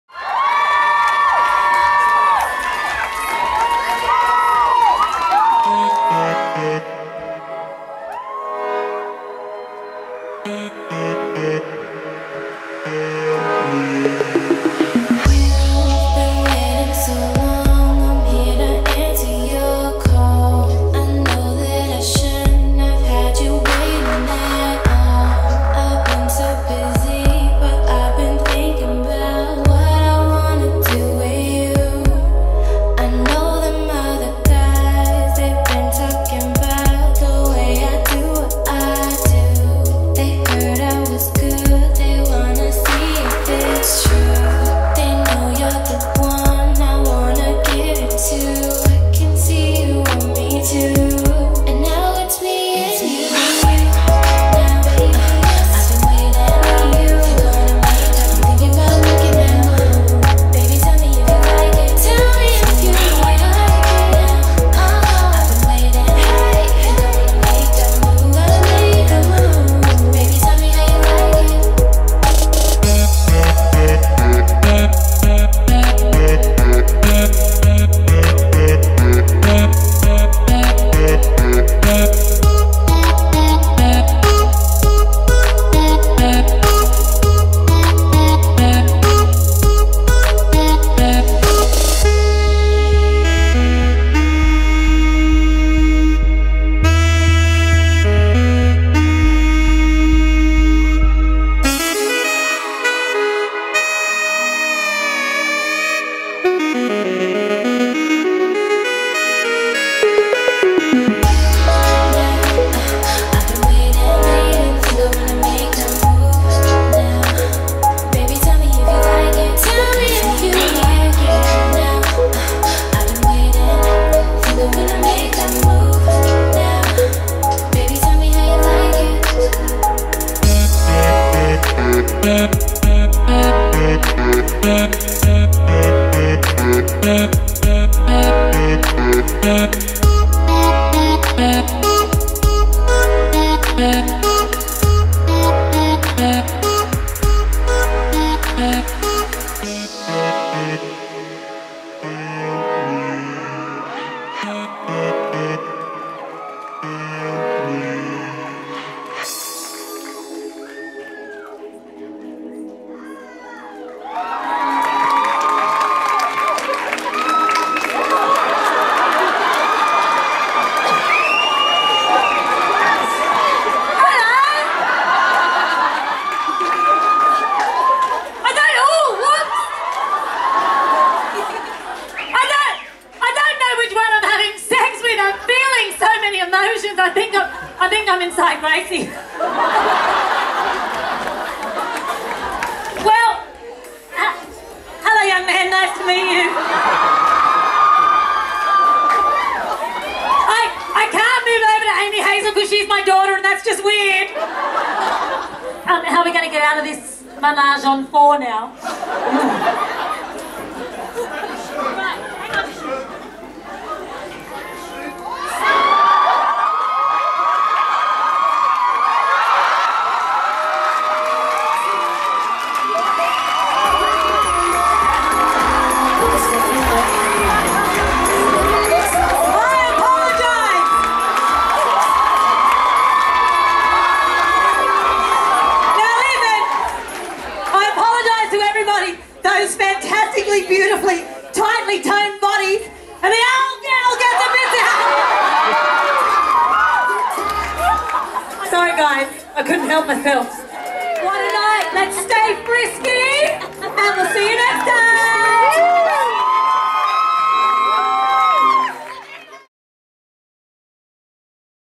FRISK 2017 - Ménage à Trois: Part II
On Saturday 21st October, 2017, 30 sexy finalists were given the opportunity to express their version of "FRISK". Our audience were in for one wild night!!!

Our opening show featured the ever so amazing Amy Hazel, Gracie Buntine and Taylor Diamond Lord!